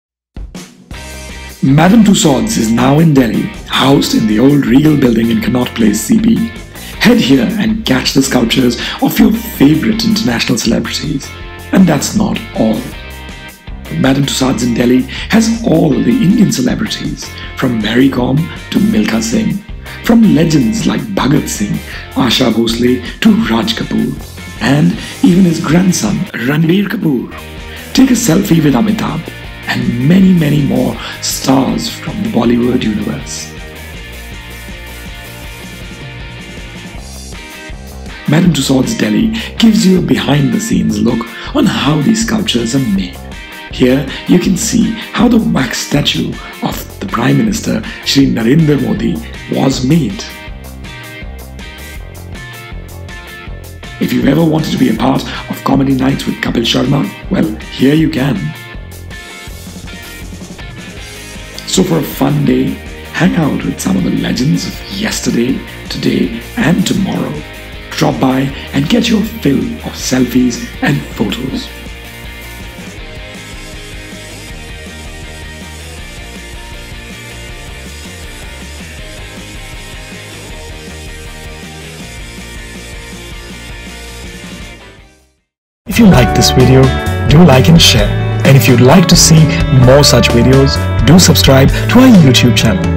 Madame Tussaud's Delhi Connaught Place New Delhi
Madam Tussauds is now in Delhi, housed in the old regal building in Connaught place CP, head here and catch the sculptures of your favourite international Celebrities
And thats not all, Madam Tussaud’s in Delhi has statues of Indian Celebs as well from  Mary Kom to Milkha Singh, from legends like Bhagat Singh, Asha Bhonsle, to Raj Kapoor or his grand son Ravir kapoor, take a selfie with Amitabh, and many many more stars from Bollywood.
Madam Tussauds, Delhi, gives you a behind the scene look on how these sculptures are made, Here you can see how the wax statue of the Prime Minister Shri Narinder Modi has was made.
Wanted to be part of Comedy nights with Kapil Sharma, well here you can, for a fun day hanging out with some of the legends of yesterday, today and tomorrow, drop by and get your fill of selfies and photos.
Where: 44 Regal Building, Connaught Place, New Delhi, Delhi 110001Timing: 10 AM to 7.30 PM, seven days a weekPhone: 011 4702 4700
Cost: 660 (Children between 3 to 11) 860 (Adults 12+), do check as they have group discounts above 5 and special offers on tickets